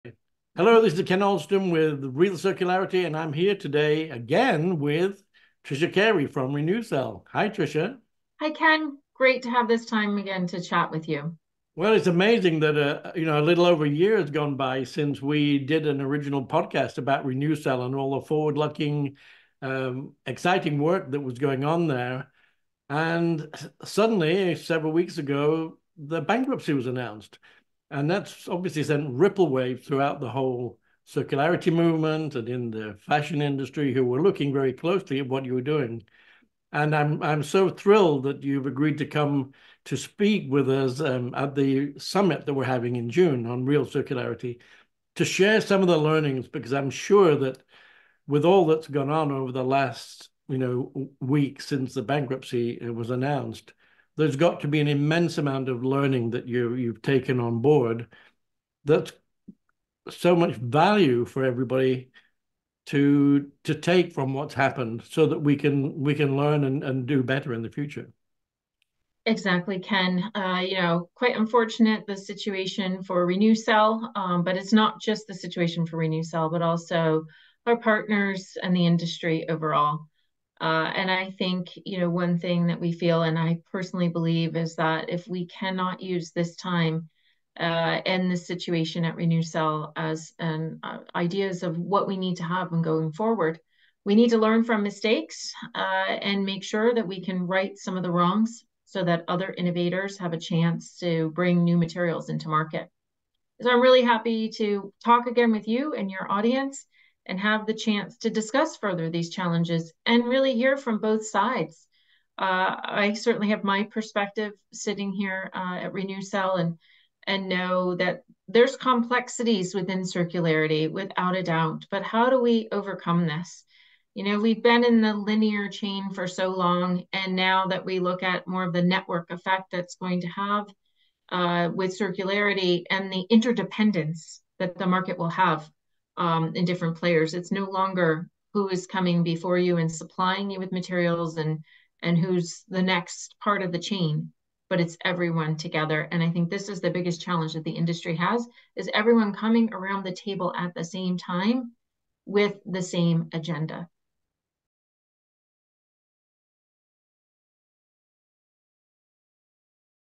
Renewcell: Lessons Learned
Trailer for the Real Circularity Summit on June 21, 2024, featuring Tricia Carey, COO of Renewcell.

Tricia previews important circular economy implementation lessons we must learn from the Renewcell bankruptcy.